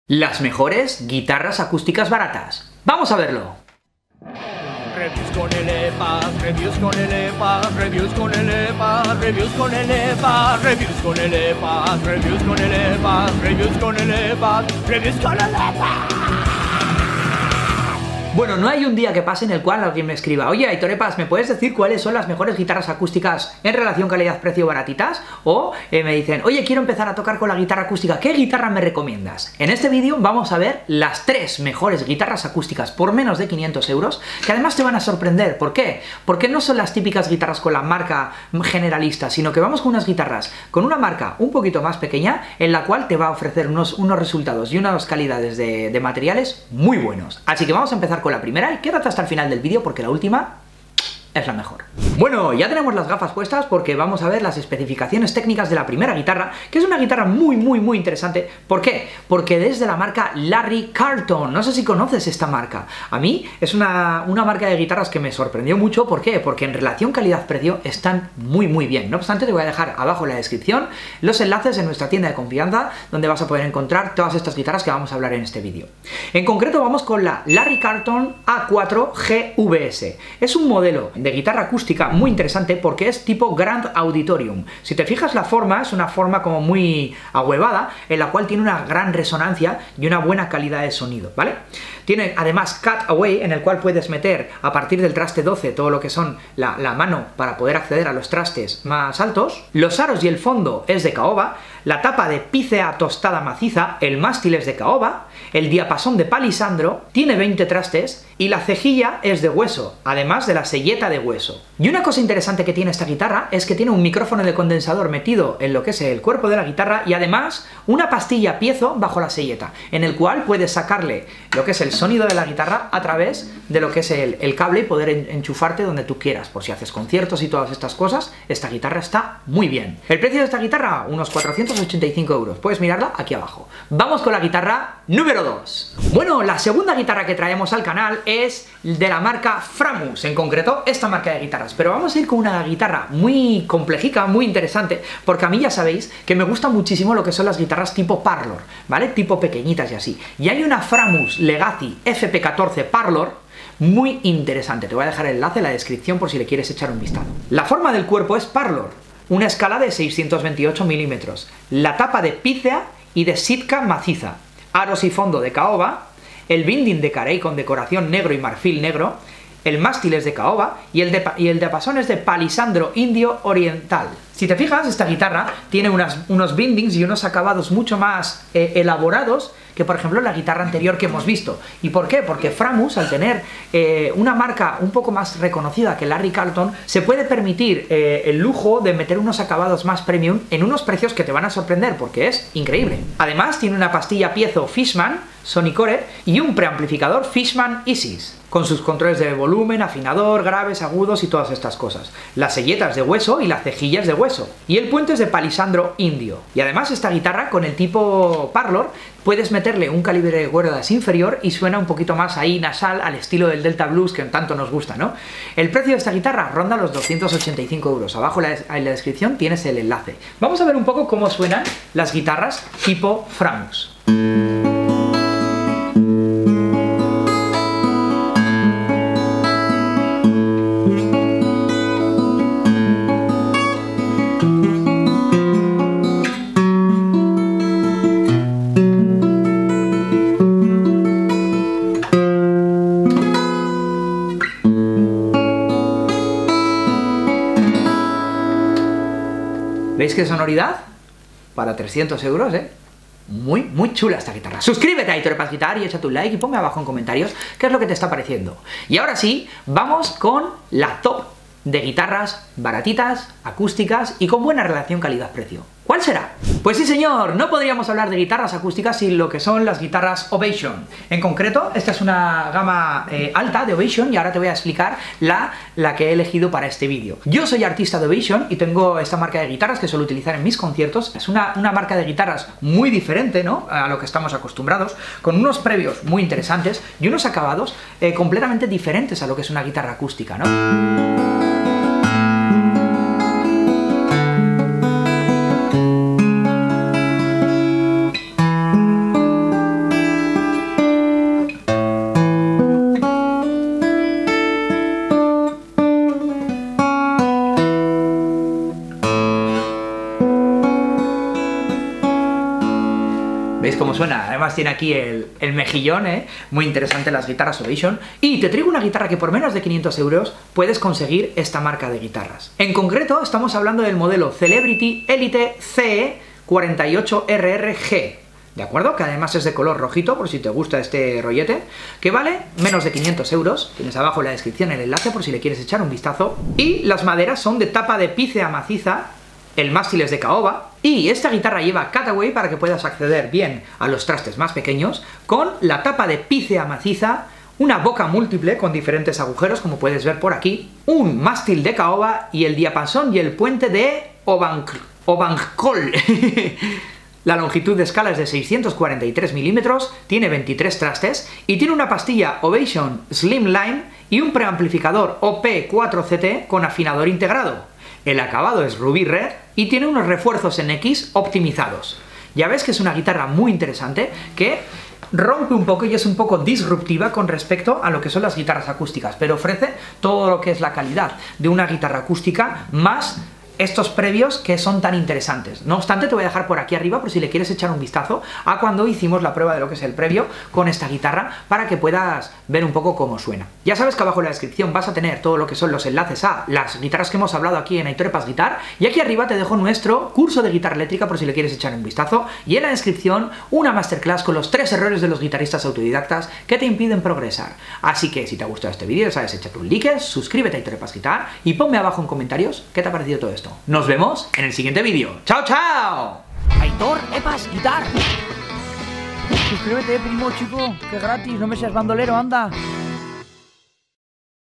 🎸 Las MEJORES Guitarras ACÚSTICAS BARATAS
🎸 ¿Buscas una guitarra acústica que se ajuste a tus necesidades con un presupuesto ajustado? En este vídeo te traigo una selección de tres guitarras acústicas con una muy buena relación calidad-precio por menos de 500€.

Quieres aprender a tocar la Guitarra?

00:00 Las MEJORES Guitarras ACÚSTICAS BARATAS
00:52 Larry Carlton A4-G VS
02:17 Framus Legacy FP 14 Parlor SV VSNT E
04:26 Ovation Celebrity Elite CE48-RR-G
06:34 Resumen